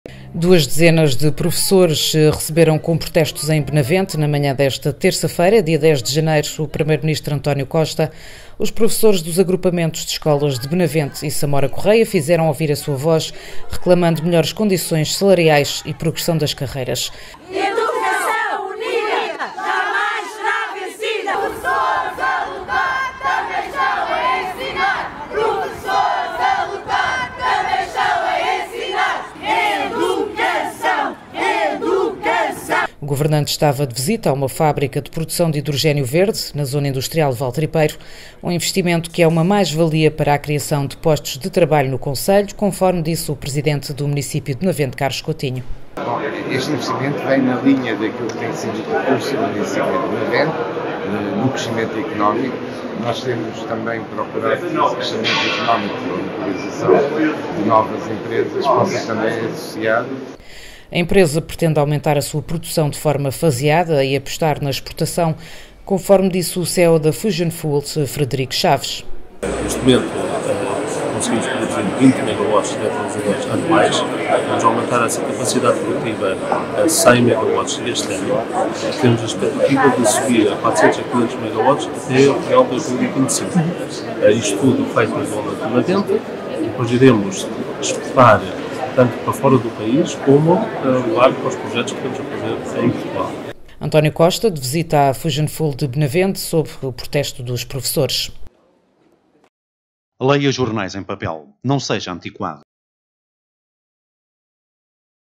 Primeiro-ministro recebido com protestos em Benavente
António Costa visitou uma das unidades de produção de hidrogénio verde

Duas dezenas de professores receberam com protestos em Benavente na manhã desta terça-feira dia 10 de Janeiro, o primeiro ministro António Costa.
Os professores dos agrupamentos de escolas de Benavente e Samora Correia fizeram ouvir a sua voz reclamando melhores condições salariais e progressão das carreiras.
O governante estava de visita a uma fábrica de produção de hidrogénio verde na Zona Industrial de Vale Tripeiro. Um investimento que é uma mais-valia para a criação de postos de trabalho no concelho, conforme disse o presidente do município de Benavente, Carlos Coutinho.